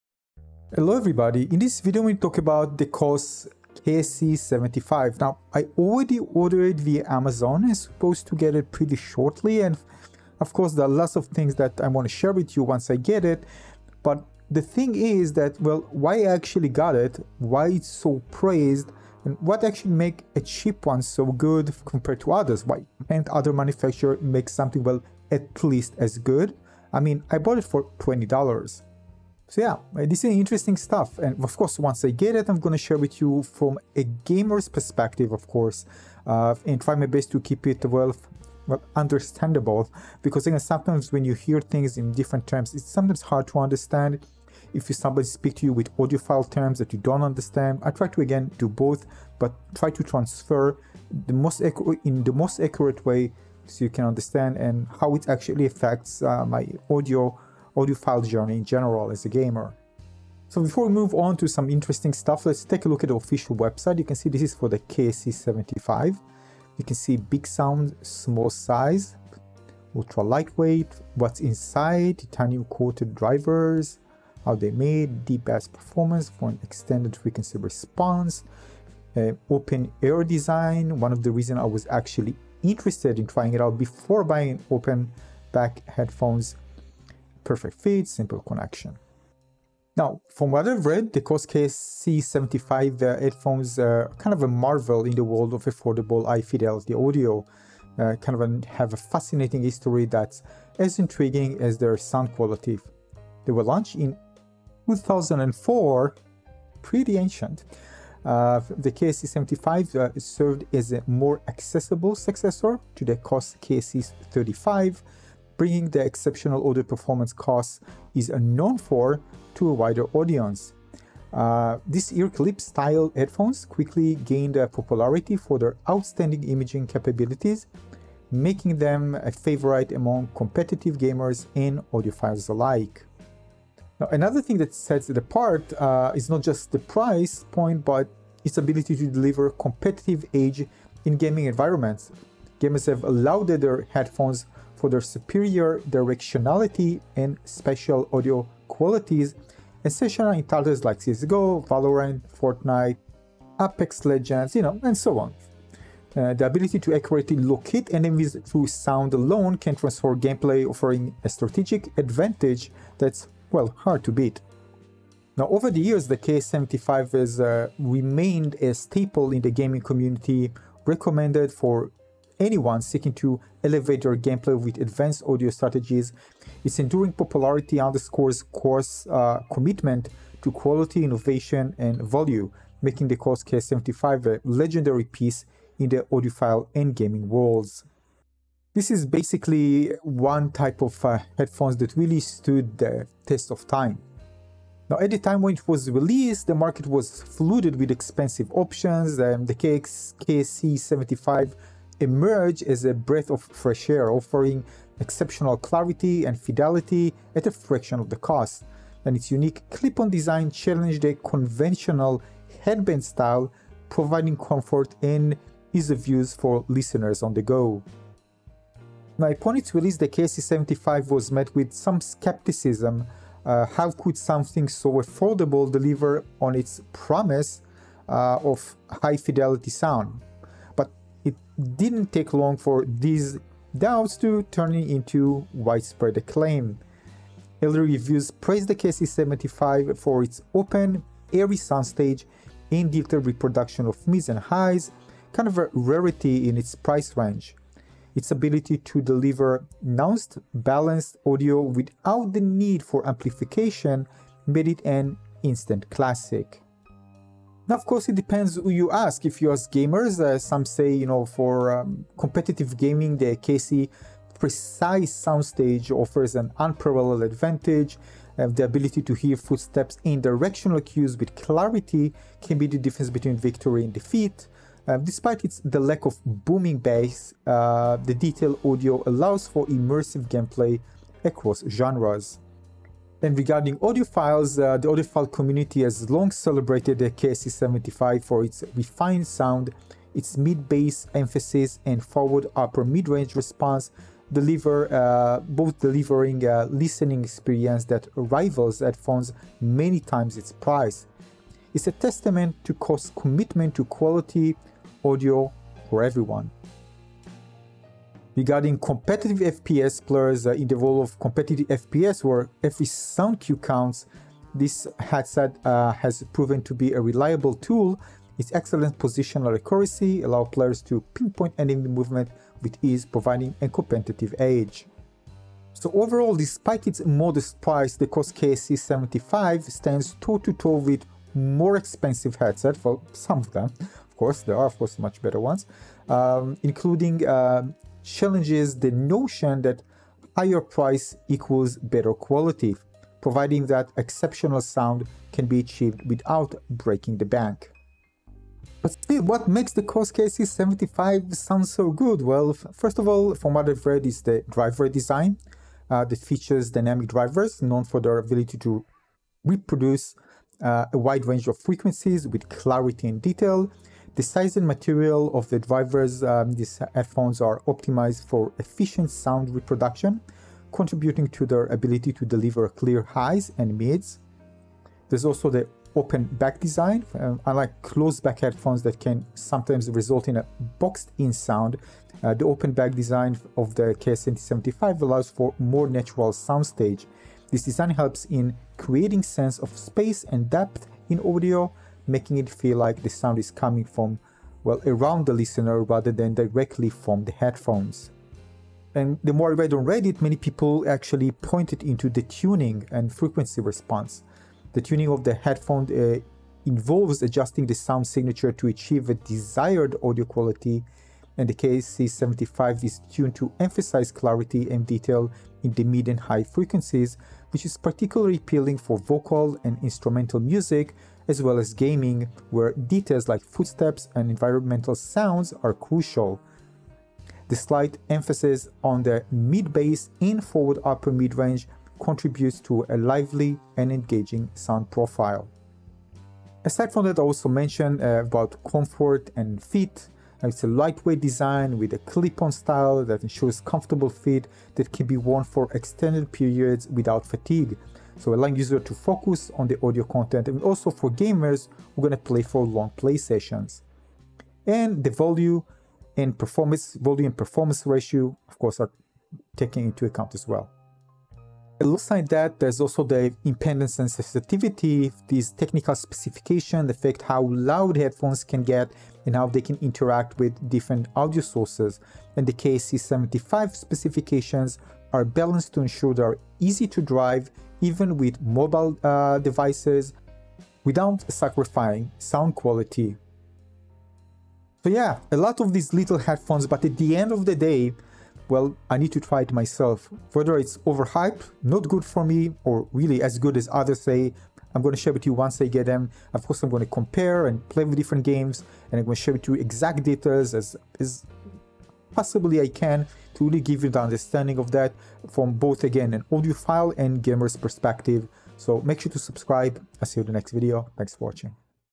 What made Koss KSC75 Legendary among Gamers & Audiophiles | A Gamer's Audiophile Journey Ep. 7
Join us in Episode 7 as we dive into the fascinating history of the Koss KSC75 on-ear clip headphones and explore the journey that catapulted them to legendary status among competitive FPS gamers and audiophiles. Discover why these affordably priced, open-back headphones are celebrated for their exceptional clarity, tonality, and comfort, making them a top choice for both music enthusiasts and gaming pros. Learn about the KSC75's unique design, superior sound quality, and how they deliver an unparalleled audio experience for gamers and music lovers alike.

Don't miss out on uncovering the secrets behind the KSC75's success and how they continue to dominate the gaming audio scene until today.

ALL EPISODES (More to come)